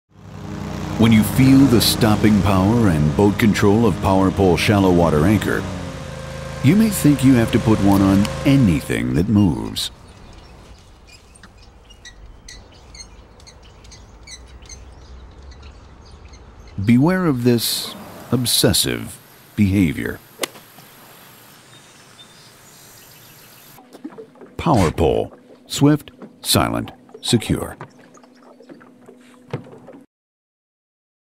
Power-Pole Obsession
New TV spot for Power-Pole shallow water anchor. Power-Pole users are crazy about this product. This commercial plays off their obsession with the stopping power of their Power-Pole shallow water anchor.